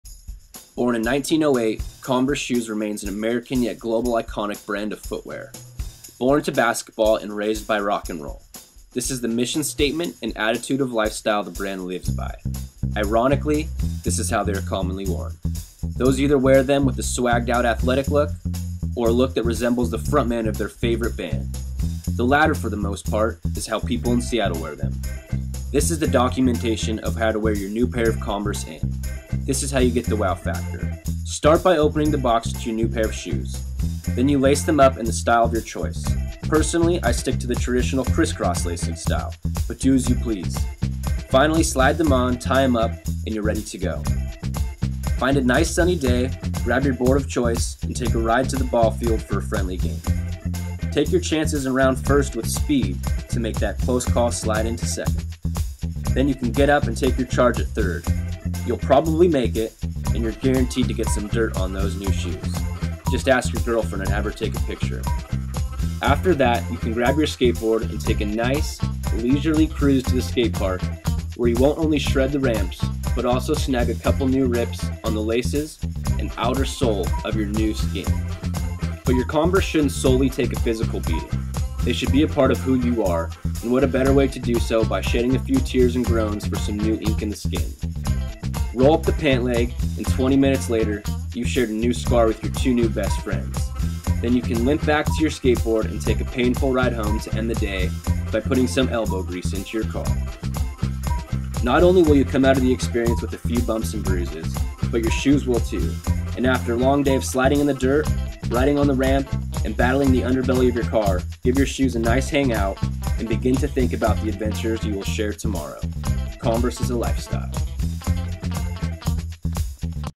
Converse is a lifestyle
My Audio Slideshow for Multimedia Principles with Dr. Peg Achterman. Converse is a Lifestyle